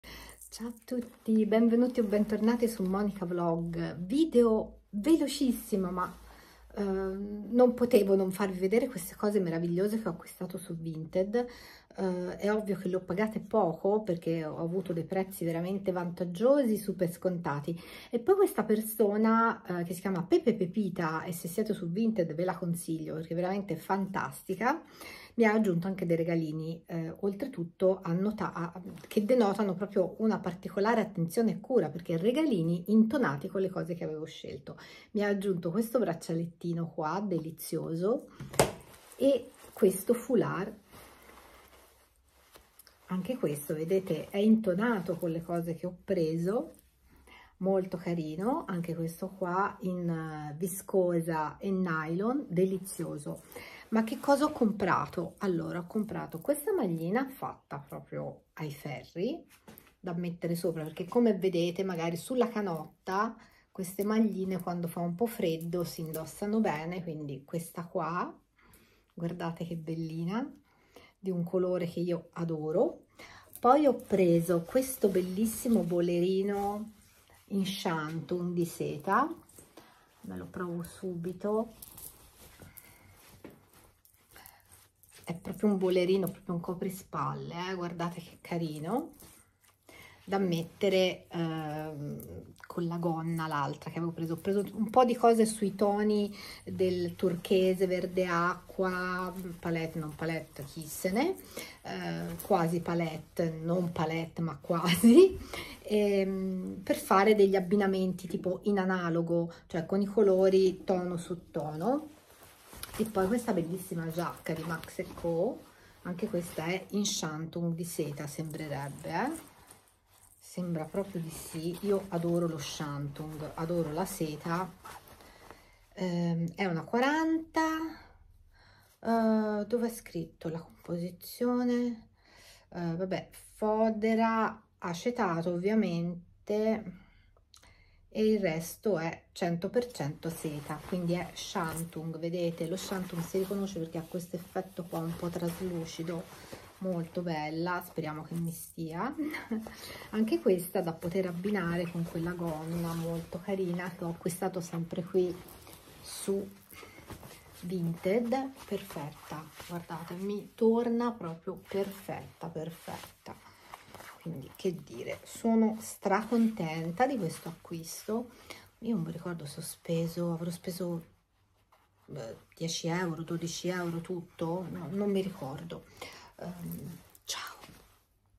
Mini TRY ON HAUL ACQUISTI BELLISSIMI su VINTED 20€
Non potevo aspettare per farvi vedere questi acquisti appena arrivati! Sono super contenta con 20 € ho preso tutte queste cose che sono tenute in modo impeccabile! Se vuoi seguirmi su Vinted ecco il